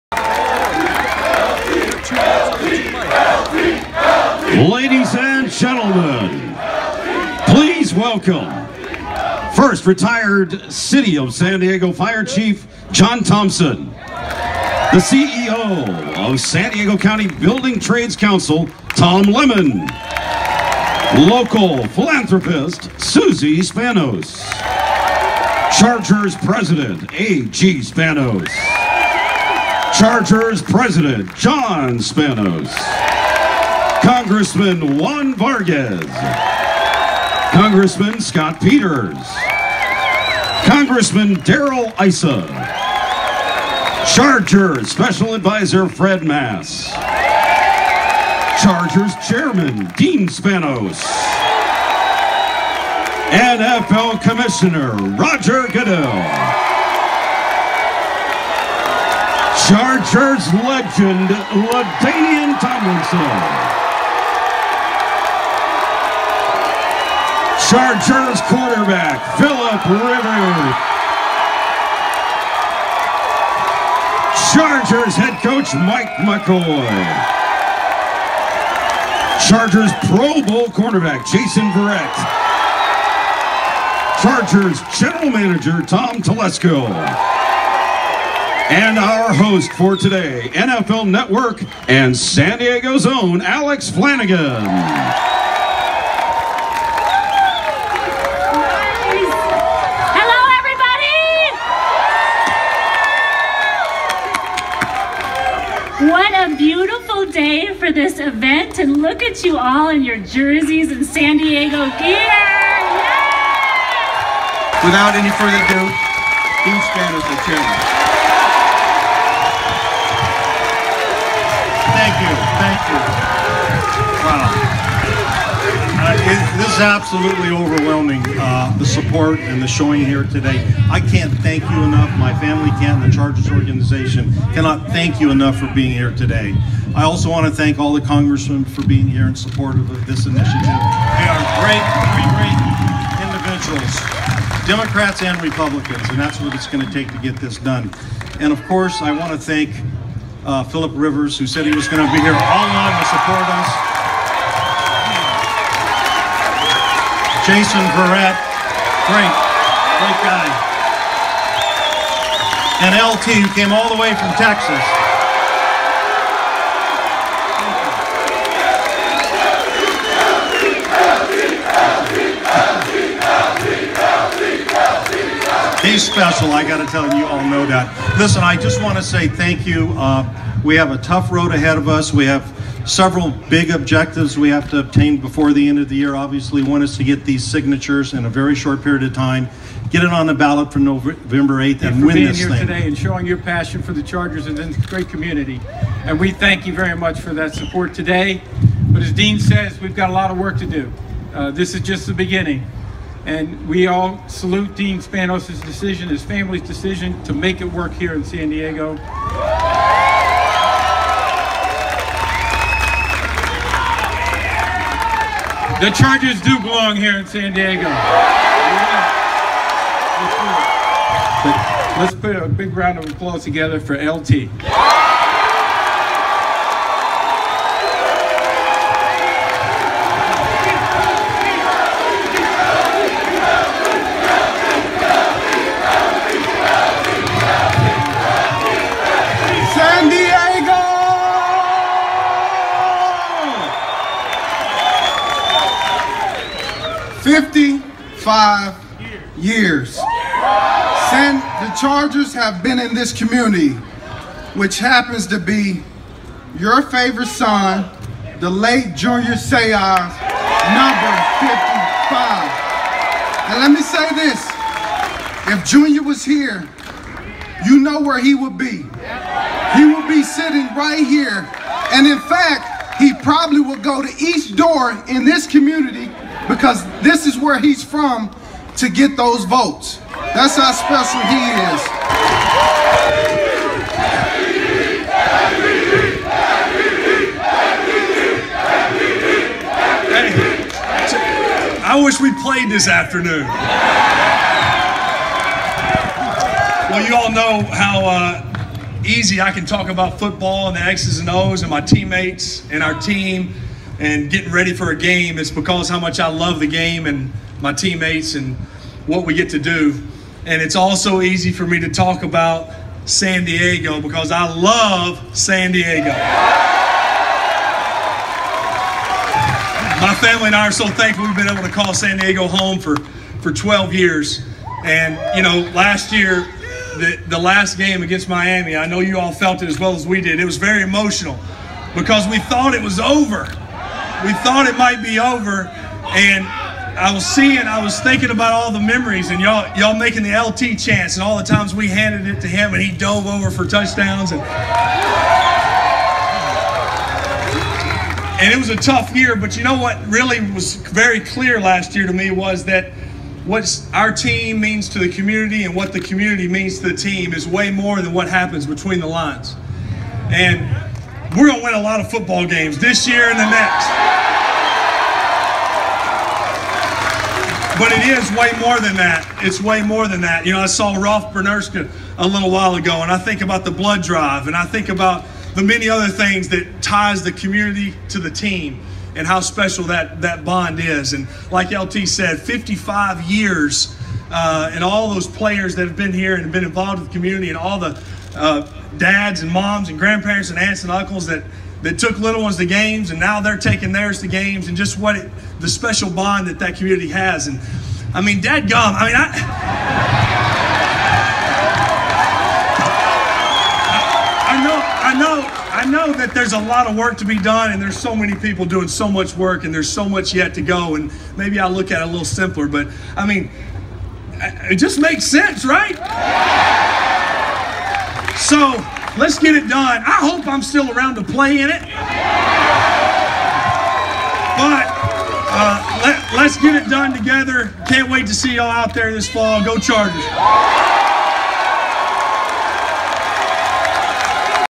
San Diego Chargers signature rally!
LaDainian Tomlinson, Philip Rivers, Dean Spanos and Roger Goodell spoke today at the Chargers signature rally!